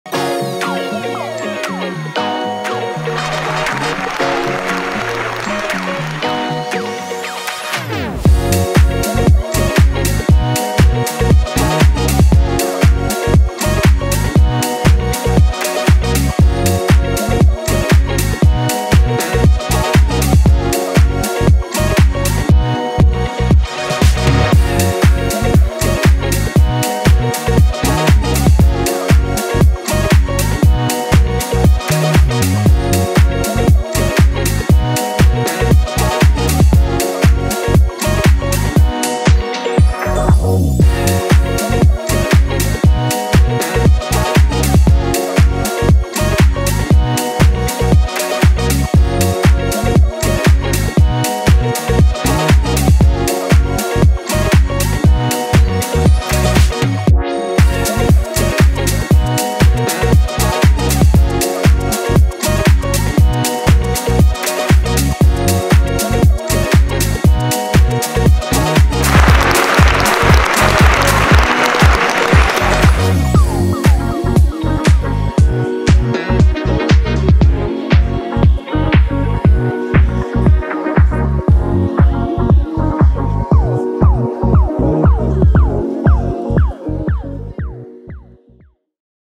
University of Zürich: Faculty of Business and Economics Graduation 2023
Filmed and edited the recap/after-movie of the University of Zurich graduation '23.